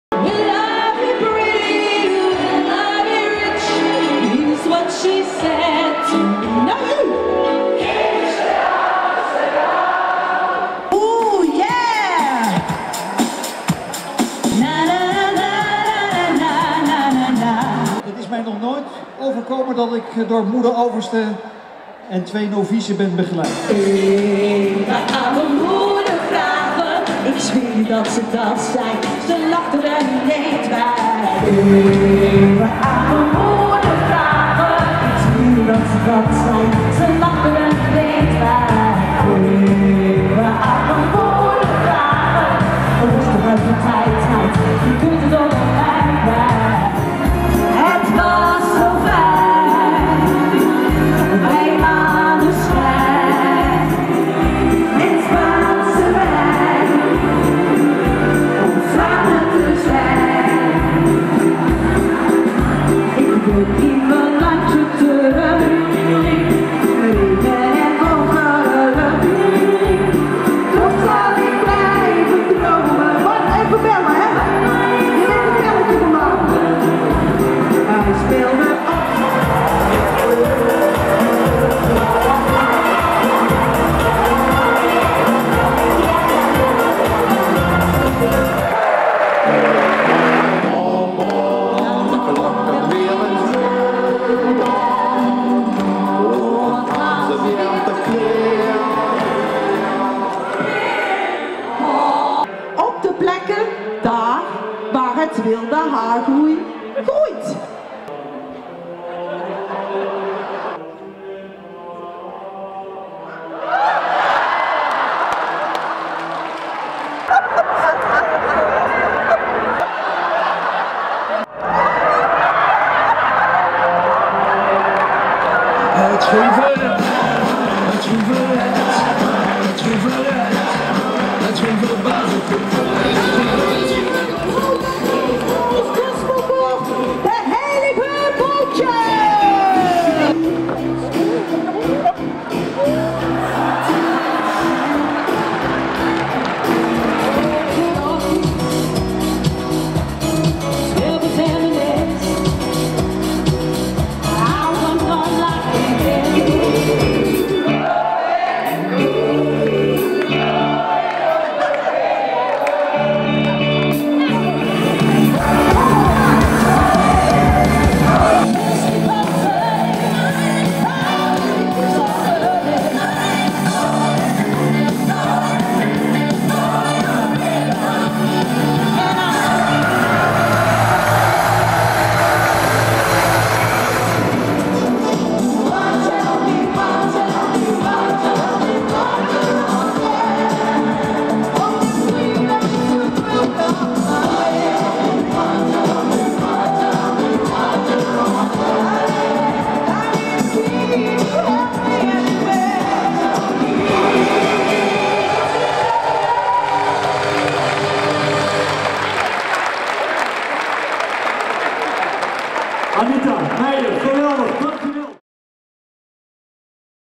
dfh18 Dreesfeest
Het Dreesfeest op vrijdagmorgen tijdens DFH2018 in de grote feesttent. Met Stampei in de Abdij en Anita Meyer